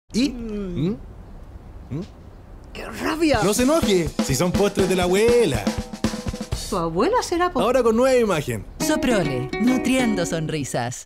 Soprole Postres de la Abuela ¡Nueva Imagen!
Si quieres un postre único, tradicional y con ese toque delicioso inspirado en las recetas caseras de las abuelitas, tienes que probar los Postres de la Abuela de Soprole ¡Ahora en su nueva imagen!